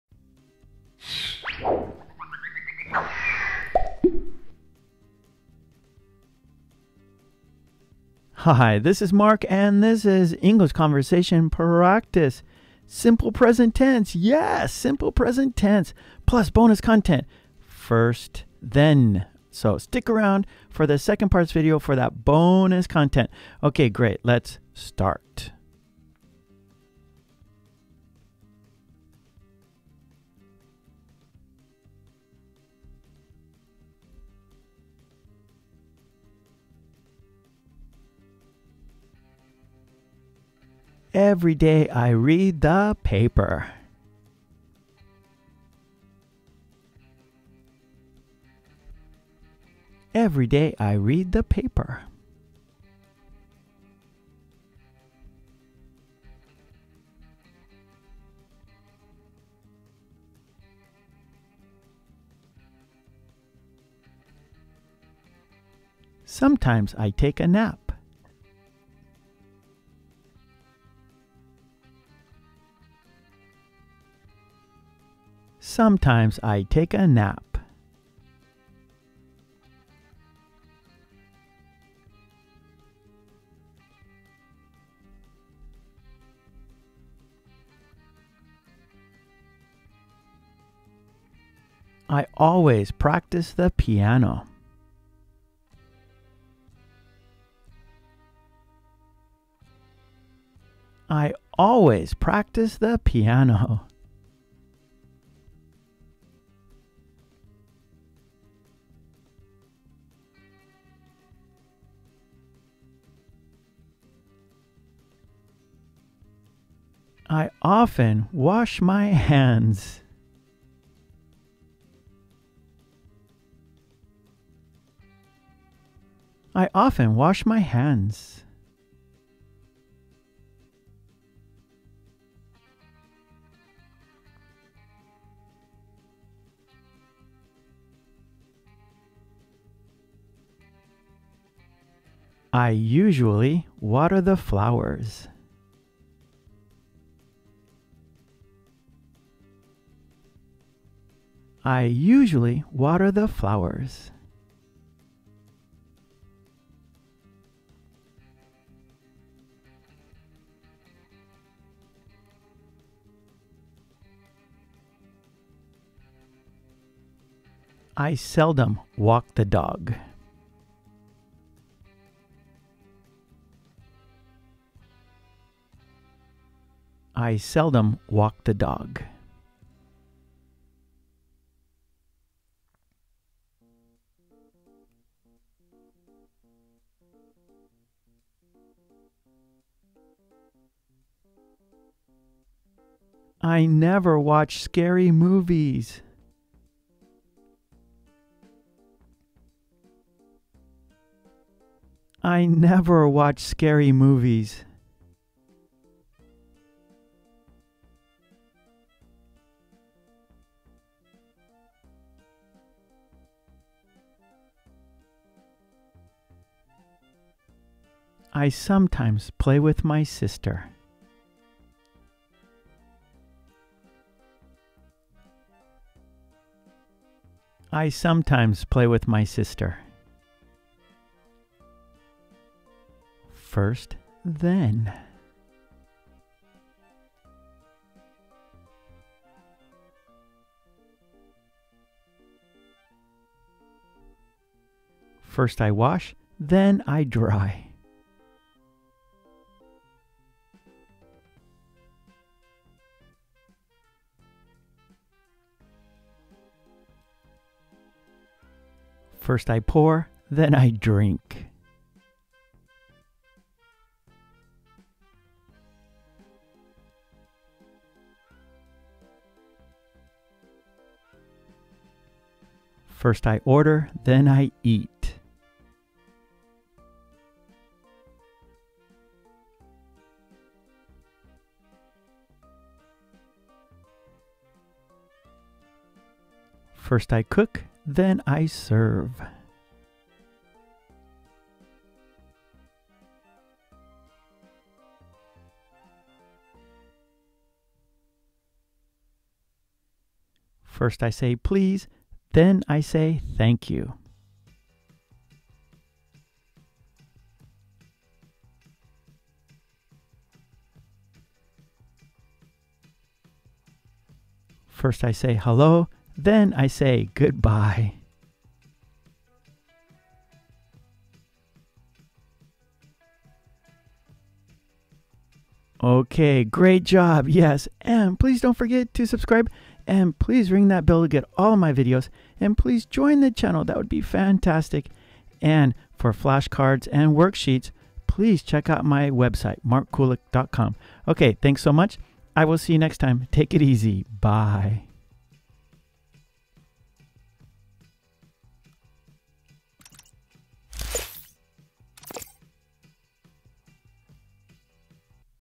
The Simple Present Tense (I + infinitive verb) | English speaking practice - Mark Kulek ESL
Focus on Grammar. Easy to Understand Patterns. Learn English with Mark Kulek ESL. Video #1 in the series. Subject + Infinitive Verb. The Simple Present Tense + First I ... then I Each conversation is illustrated and color coded for better understanding. This is an interactive listening and speaking video. Please listen and then repeat after Mark. Have fun learning English and speak English now.

⏱TimeStamps⏱
0:00 Intro
0:29 The Simple Present Tense
3:53 First I ... then I ... (pattern)

English in use:
Every day I read the paper. / Sometimes I take a nap. / I always practice the piano. / I often wash my hands. / I usually water the flowers. / I seldom walk the dog. / I never watch scary movies. / I sometimes play with my sister.
First I ... then I ... : First I wash then I dry. / First I pour then I drink. / First I order then I eat. / First I cook then I serve. / First I say please then I say thank you. / First I say hello then I say goodbye.

▶️ LiveStream Classes every *Tuesday at 5 p.m. and Friday at 10 a.m. - all times are (Japan Asia/Tokyo)*